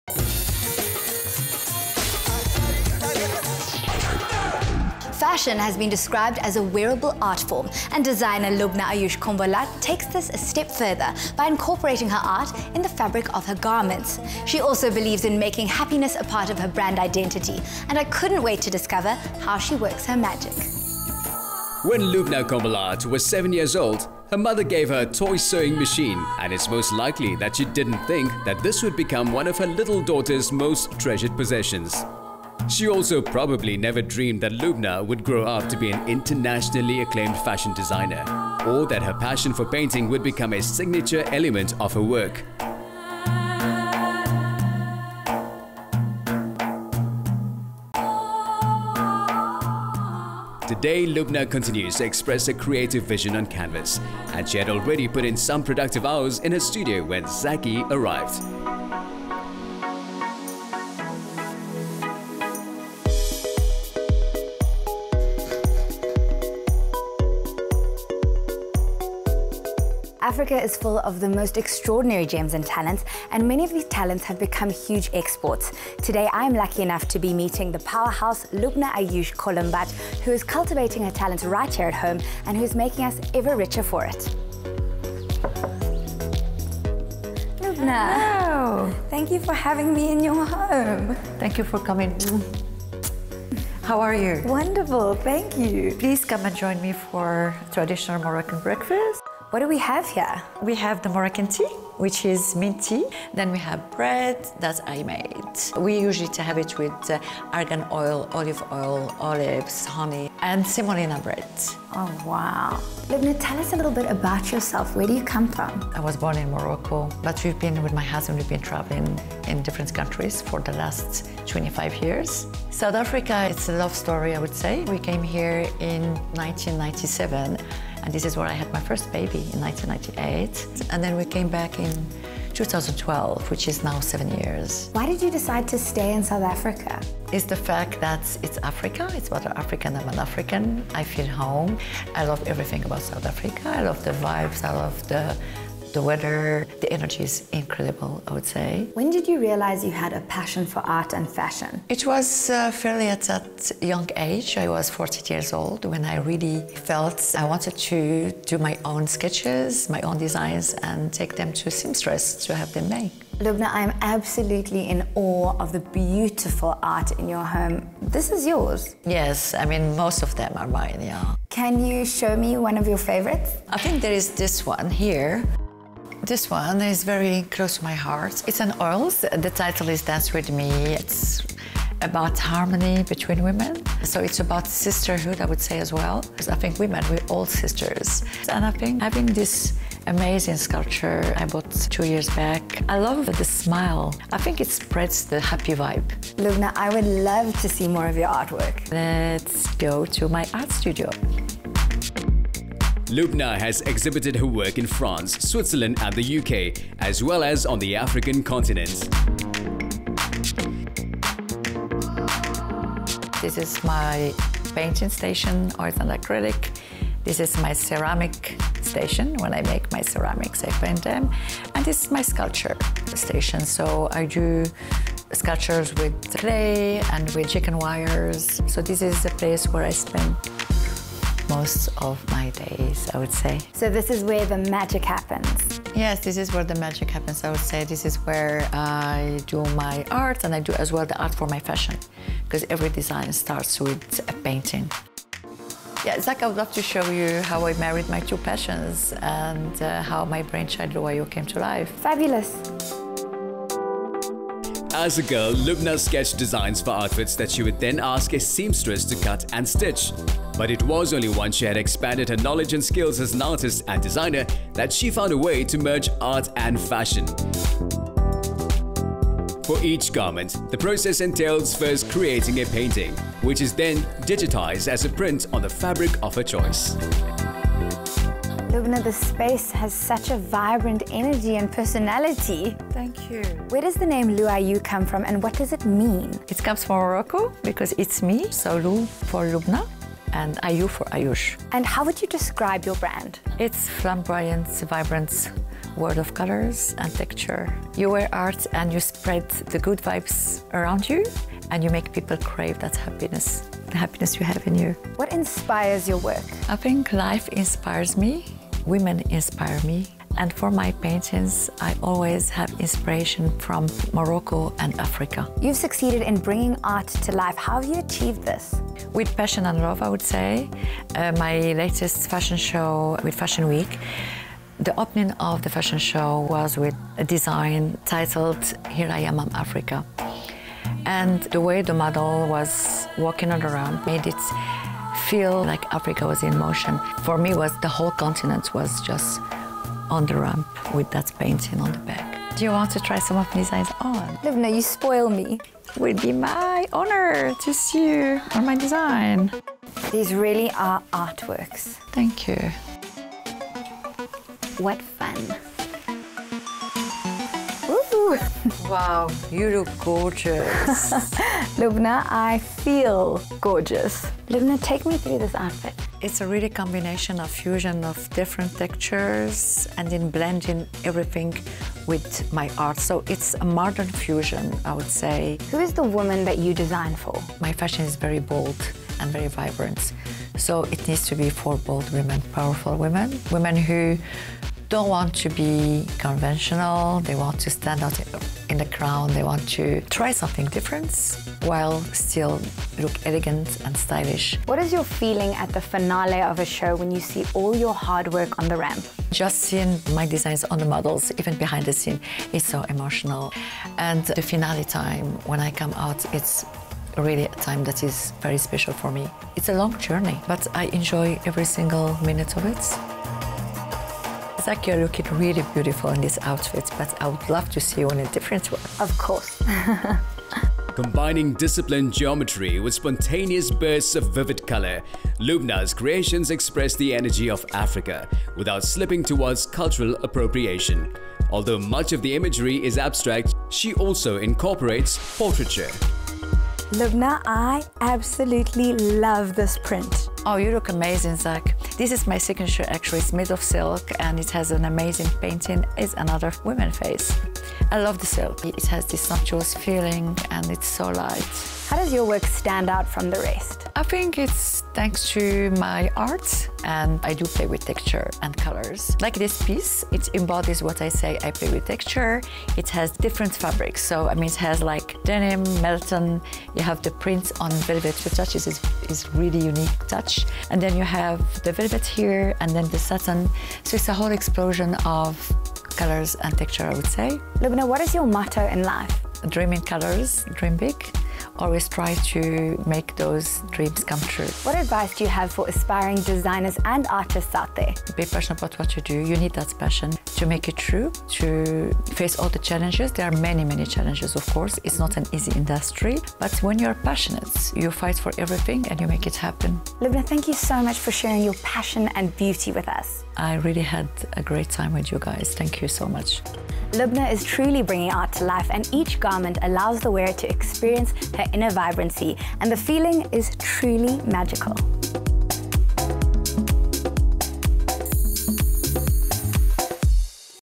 At Home With Fashion Designer Loubna Combalat
Fashion has been described as a wearable art form – and designer Loubna Combalat takes this a step further – by incorporating her art in the fabric of her garments.